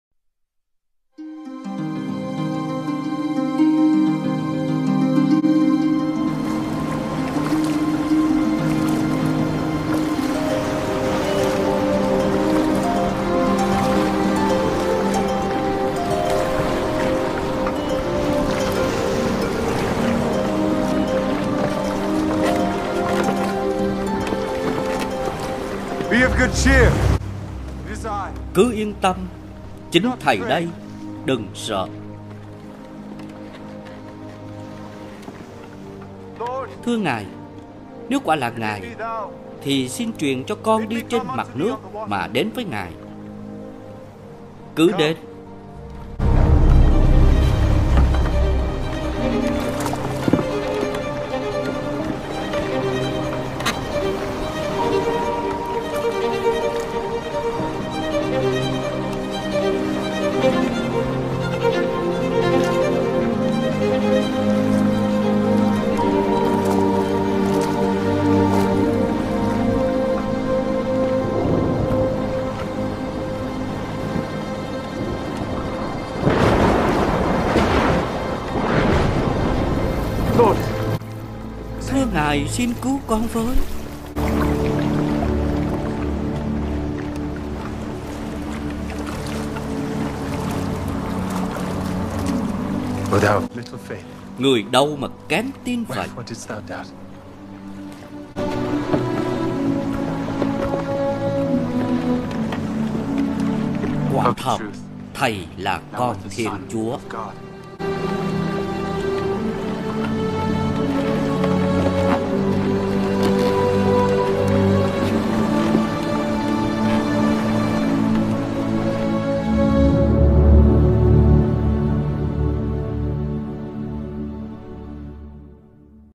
Chúa Giêsu: “Người gì mà kém tin vậy?”
Chúa Giêsu sẽ hỏi chúng ta vào thời điểm mà chúng ta đối diện với sóng gió cuộc đời như Phêrô hôm nay sắp chìm xuống biển.

Chúng ta tự hỏi mình: “Điều gì đang làm cho tôi sợ hãi?”